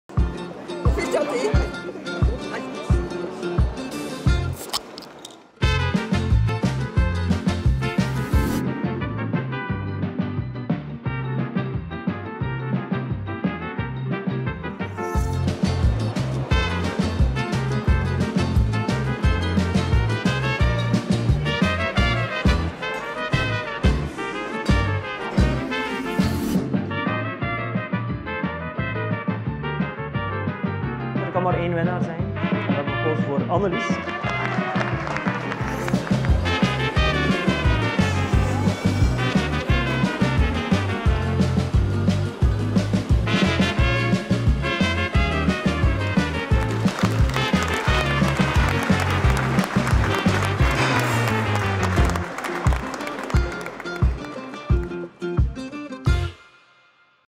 De Smaakmaker VANRSL verwent 500 fijnproevers op het stoofvleesfestijn
Roeselare, stad van de voeding, lanceerde voor de Week van de Smaak (21 -27 november 2022) een programma om van te smullen. De blikvanger in het Roeselaarse programma was het grote stoofvleesfestijn in de Paterskerk. Daar genoten 500 aanwezigen van stoofvlees klaargemaakt volgens het recept van Annelies Mesure, de Smaakmaker VANRSL. Zij werd gekroond door een vakjury onder leiding van driesterrenchef Tim Boury. De helft van de opbrengst van het festijn gaat bovendien naar vzw De Sociale Kruidenier.